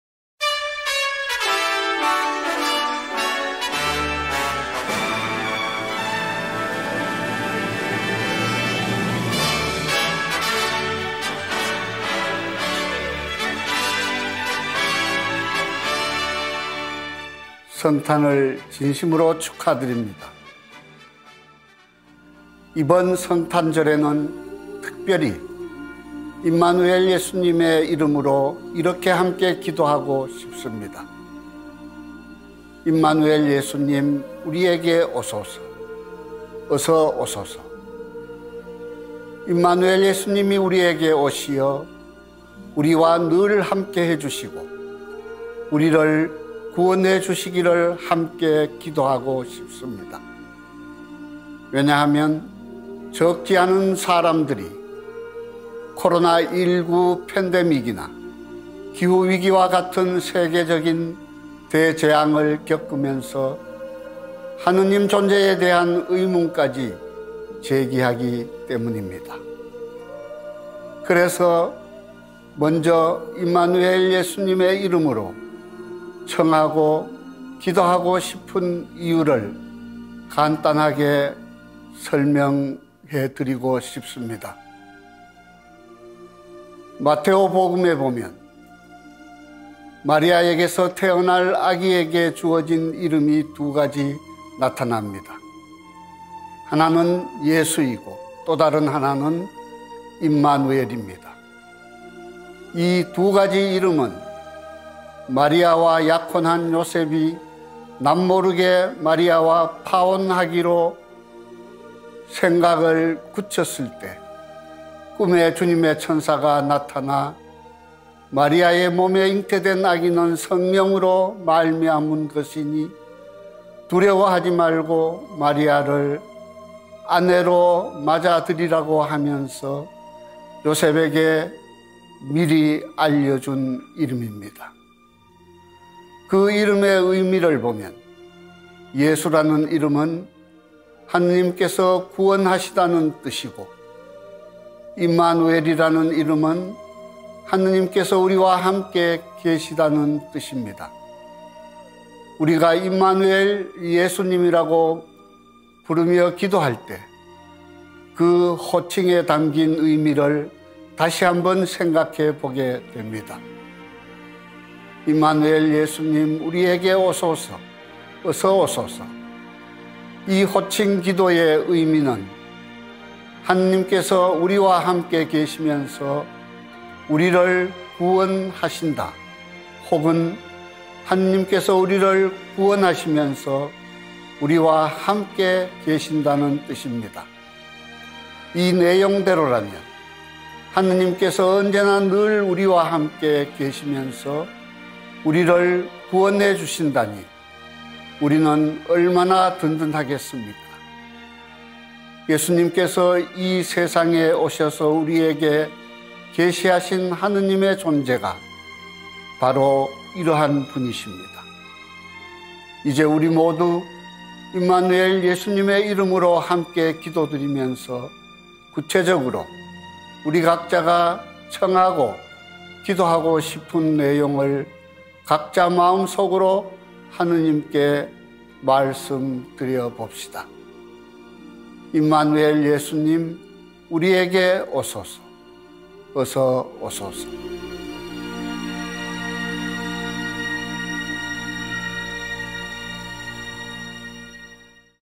안동교구장 권혁주 요한크리소스토모 주교의 '2021 성탄 메시지' - 예수님께서는 우리에게 참된 행복을 선사하기 위해 오셨습니다.
[2021 성탄 메시지 - 안동교구장 권혁주 요한크리소스토모 주교]
- 예수님께서는 우리에게 참된 행복을 선사하기 위해 오셨습니다.

안동교구장 권혁주 요한크리소스토모 주교께서
2021년 성탄 메시지를 보내주셨습니다.
함께 읽어볼까요?

방송선교ARS후원 :
후원문의, 매일미사 지향 신청 : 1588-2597
카카오톡 플러스친구 : cpbc TV